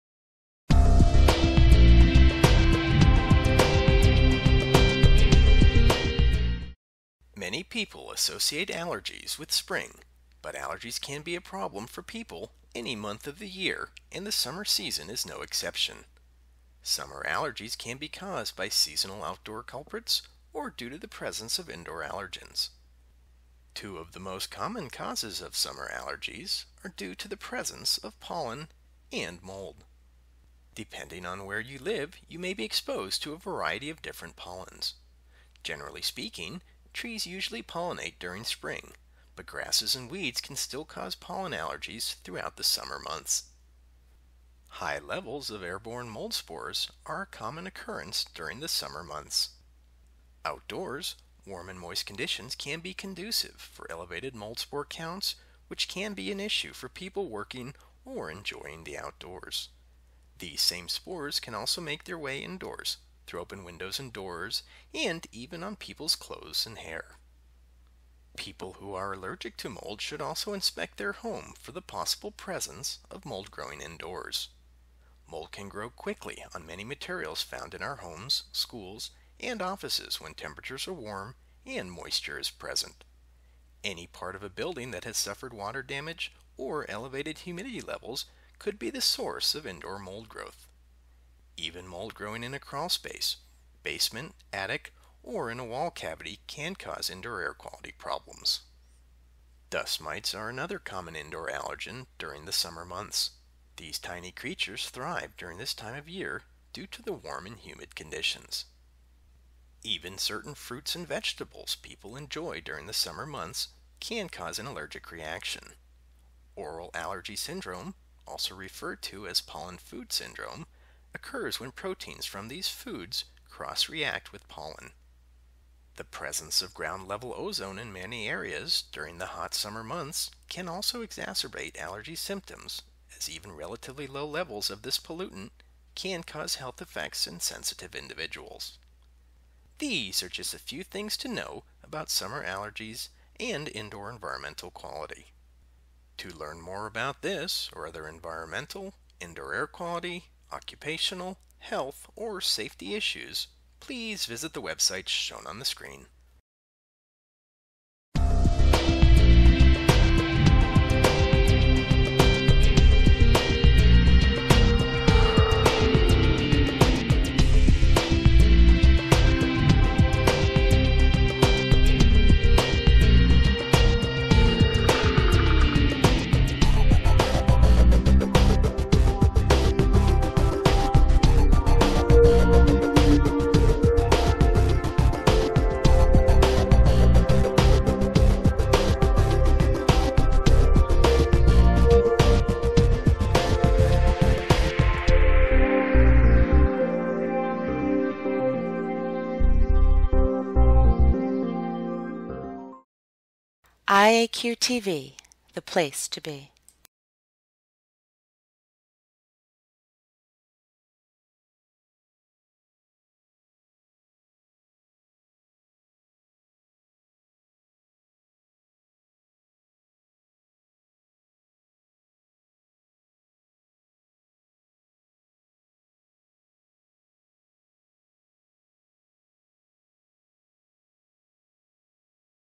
Summer Allergies
Many people associate allergies with spring, but allergies can be a problem for people any month of the year and the summer season is no exception. Summer allergies can be caused by seasonal outdoor culprits or due to the presence of indoor allergens.  Two of the most common causes of summer allergies are due to the presence of pollen and mold.

Depending on where you live, you may be exposed to a variety of different pollens.  Generally speaking, trees usually pollinate during spring, but grasses and weeds can still cause pollen allergies throughout the summer months.

High levels of airborne mold spores are a common occurrence during the summer months.  Outdoors, warm and moist conditions can be conducive for elevated mold spore counts which can be an issue for people working or enjoying the outdoors.  These same spores can also make their way indoors through open windows and doors and even on people’s clothes and hair.

People who are allergic to mold should also inspect their home for the possible presence of mold growing indoors.  Mold can grow quickly on many materials found in our homes, schools and offices when temperatures are warm and moisture is present.  Any part of a building that has suffered water damage or elevated humidity levels could be the source of indoor mold growth.  Even mold growing in a crawl space, basement, attic or in a wall cavity can cause indoor air quality problems.

Dust mites are another common indoor allergen during the summer months.  These tiny creatures thrive during this time of year due to the warm and humid conditions.

Even certain fruits and vegetables people enjoy during the summer months can cause an allergic reaction. Oral allergy syndrome, also referred to as pollen-food syndrome, occurs when proteins from these foods cross-react with pollen.

The presence of ground level ozone in many areas during the hot summer months can also exacerbate allergy symptoms as even relatively low levels of this pollutant can cause health effects in sensitive individuals.

These are just a few things to know about summer allergies and indoor environmental quality.  To learn more about this or other environmental, indoor air quality, occupational, health or safety issues, please visit the websites shown in the video or below.